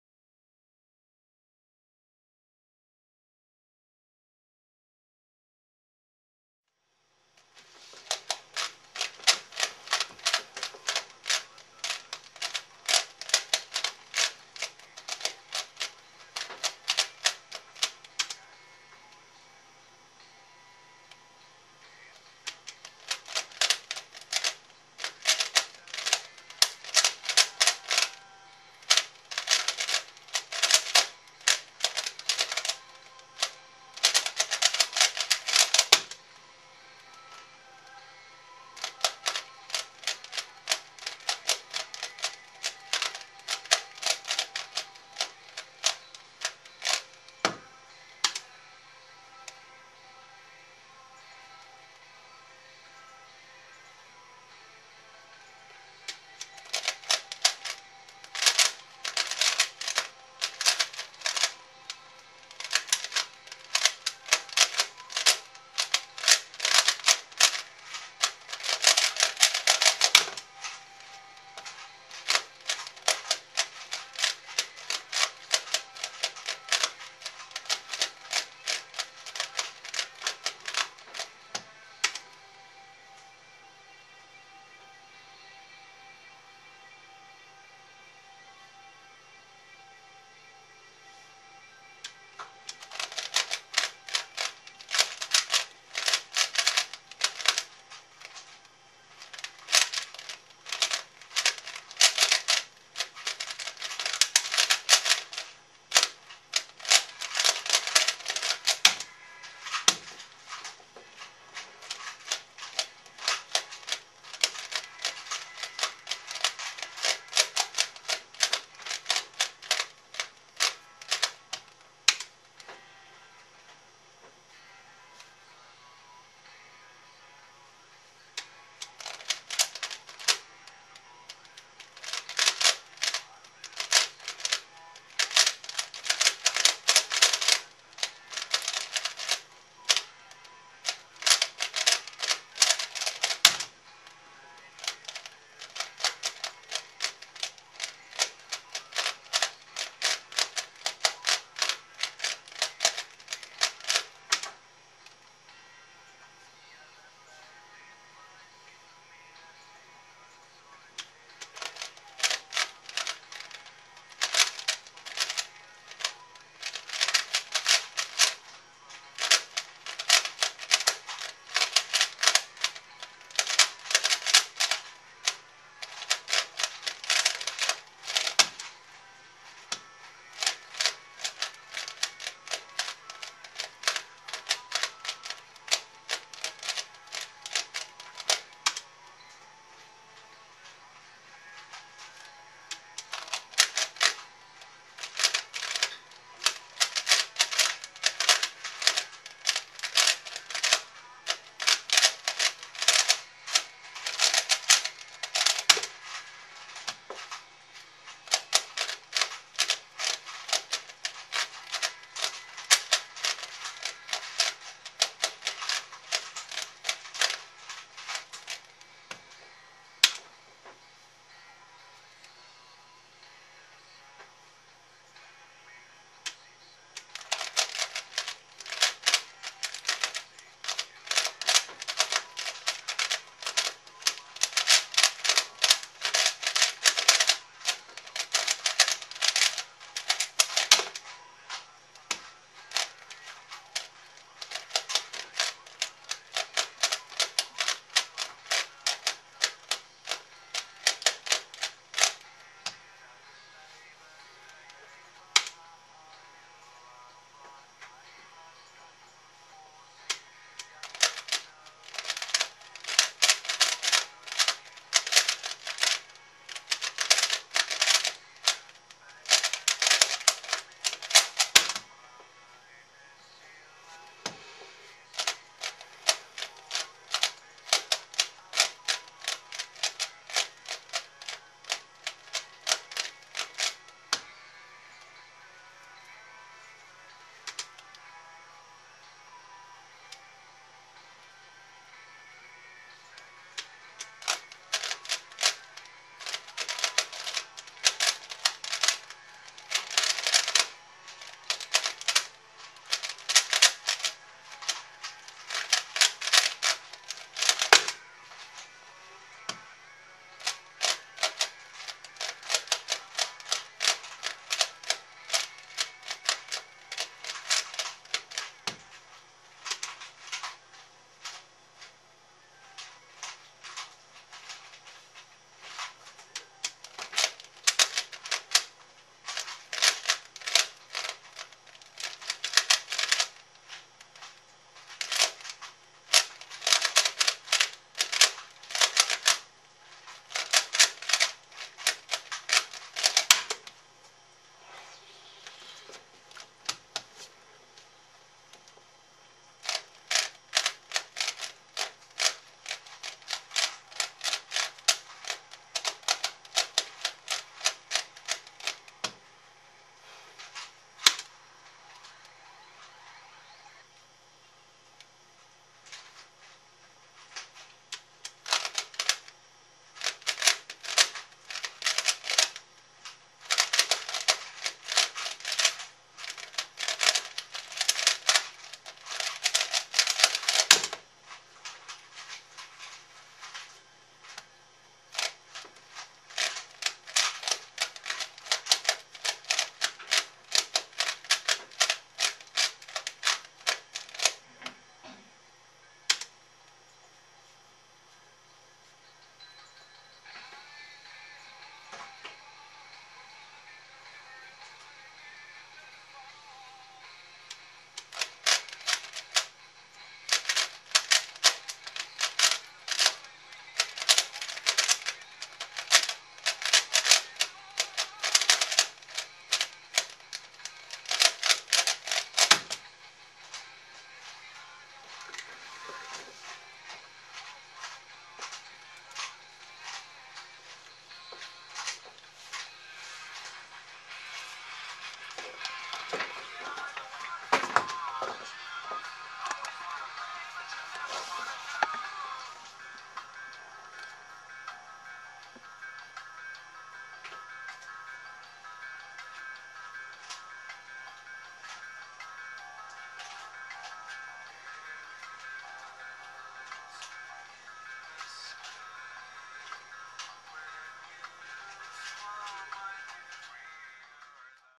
14.18 sec average of 10/12 3x3x3
3 bad solves ruined the sub-14 average.
Oh well, this is the best taped avg I've done so far.

Cube: C4U with bright Cubesmith stickers.

Average: 14.18
Standard Deviation: 1.30
Best Time: 10.99
Worst Time: 17.02
Individual Times:
1.13.26B2 D2 L R' D' F2 U2 B' F2 L2 B U2 L2 B2 F2 L2 R' D' U' L U L' D' U2 F2
2.13.75B2 F2 L' B F' L R' B2 R' D' U B F2 R B' F D' U2 R D B F' U F L2
3.16.16D2 U B2 U B R2 F' D' R2 B2 D' U R' F2 D' U2 L2 R2 D' R' F R B2 F2 L'
4.14.64U' L B' D U F' R' B U' B' F D U R2 D2 L D' F2 L' R2 U F2 L' D2 F2
5.(17.02)L' R' B2 D L' R B L' D' B2 F2 U2 L' R2 F R B2 R2 F2 L R2 B2 F U R2
6.12.74L2 R2 B F L B' F' U' F2 D' B F2 L' B' F2 D U' L' F D' U2 L' B F R'
7.13.76B2 F2 D2 U2 L2 B2 F' D2 U2 L' F2 D2 U' B L R2 B2 L R2 B R' B F2 U' L
8.(10.99)L2 B' L' B2 F' R2 F' U R U2 B2 F2 U L R2 D' U B' D L' D' B2 D' B' L2
9.13.76U2 L' B' L R2 U2 B2 U L2 R B F' D U L2 D' U R' D' F' D B F2 L R2
10.16.68U' L2 D U B R' D2 L D' B' L2 R' U2 F' L2 F' R' F R B F' R2 D F' R
11.12.47L2 R2 F2 L R2 U' L' R2 B R' B' R2 D' U2 R2 U' R' D2 U B2 D U2 F' L R'
12.14.56L2 R2 F' R2 D' L R U' B2 F D' U2 F' D2 B2 F2 D F U2 L2 R2 B' D2 F D'